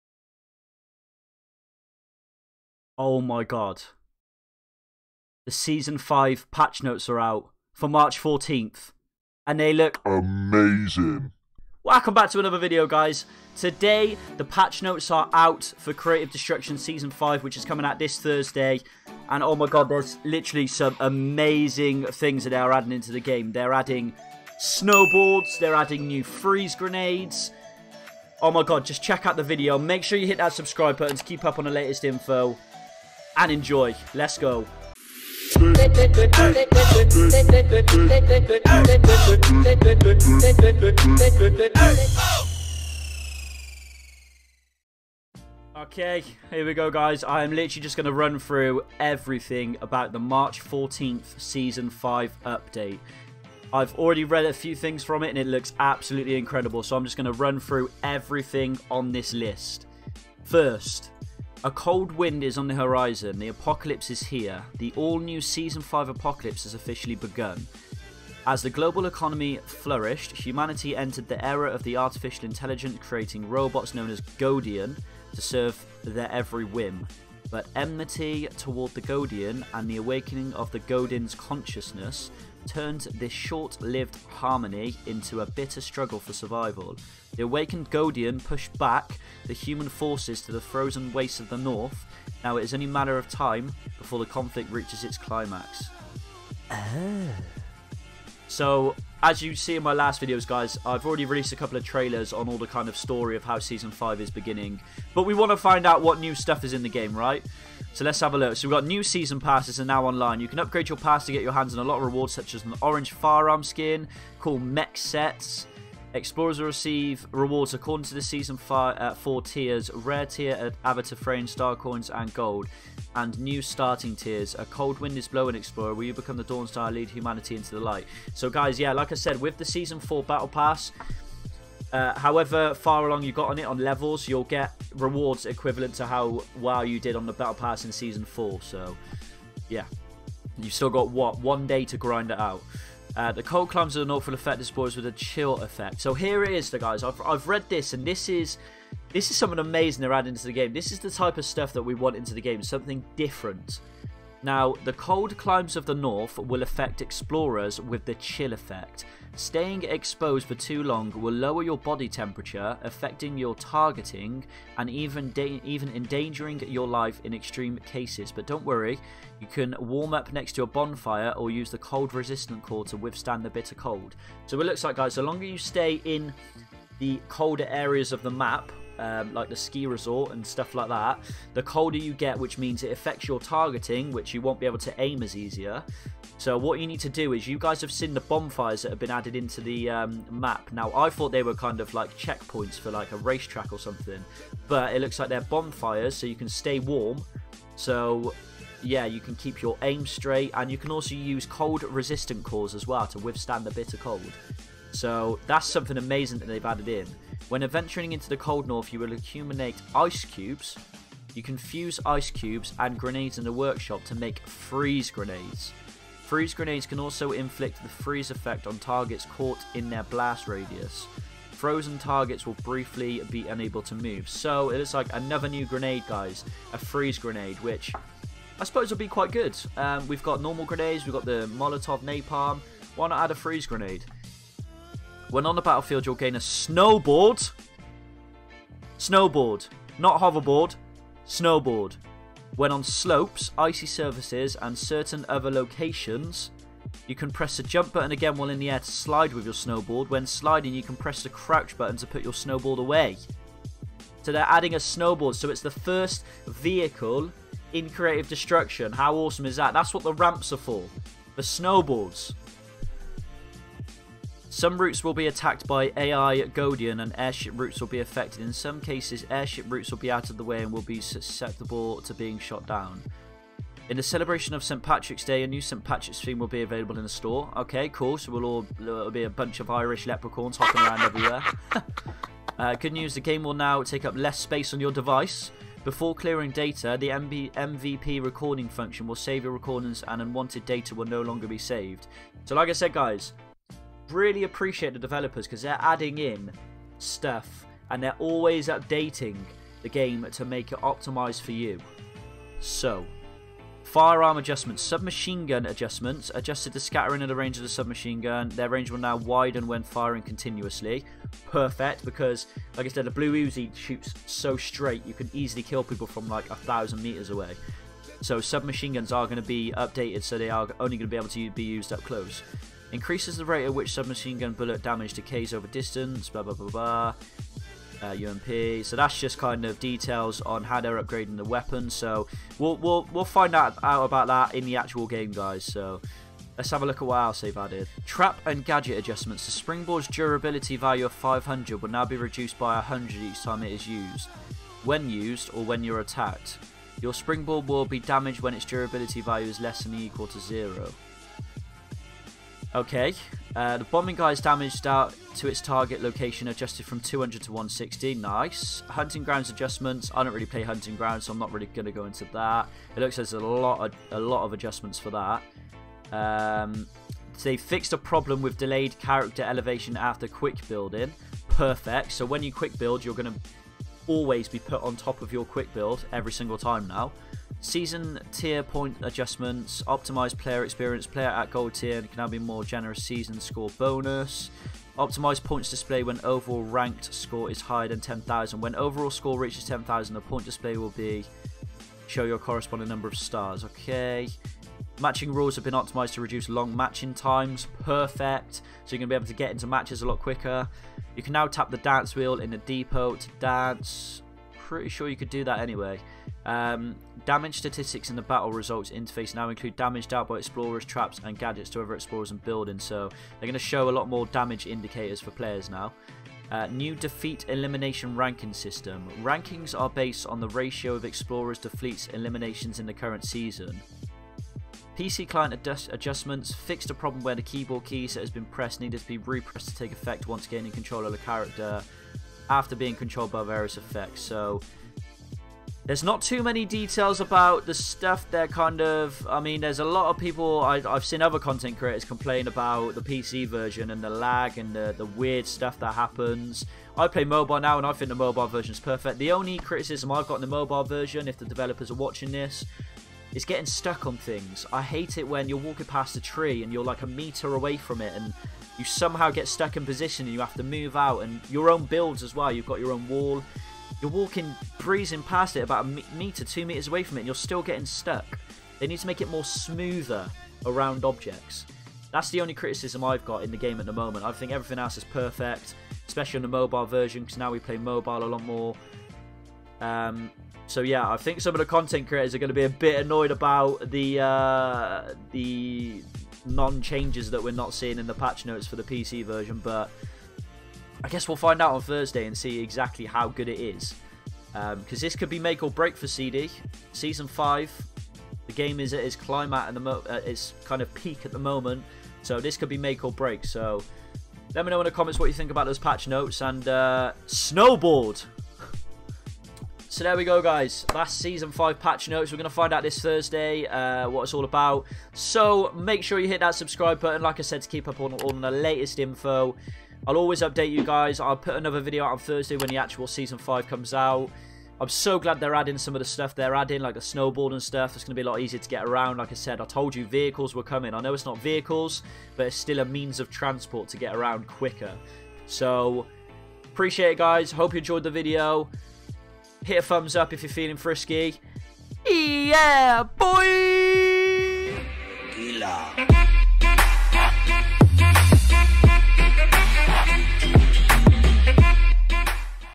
SEASON 5 FULL DETAILS!! 😲 | Creative Destruction
In this video I go over the patch notes for Season 5!!!

Thanks!

Creative Destruction
CD
Mobile Gaming
battle royale